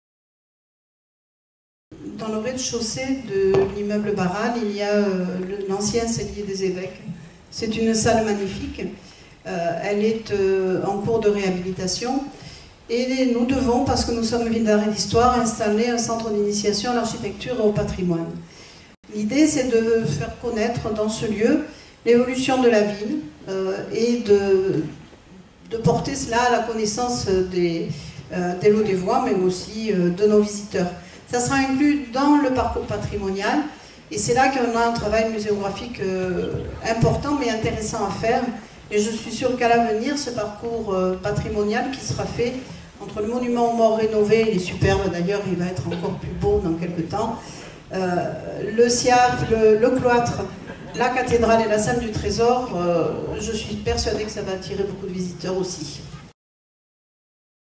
Lodève 2014 : un parcours patrimonial dans une ville d'art et d'histoire
A retrouver dans le programme de campagne de l'équipe Lodève en Mouvement avec Marie-Christine Bousquet :

- Réaliser les tranches pluriannuelles de travaux de restauration de la cathédrale.
- Aménager un circuit patrimonial reliant le cloître, la cathédrale, la salle du trésor, le cellier des évêques, le monument aux morts, le château de Montbrun et le musée  (collections Dardé et archéologiques)
- Ouvrir à tous le site du château de Montbrun, comme espace naturel et archéologique en cœur de ville.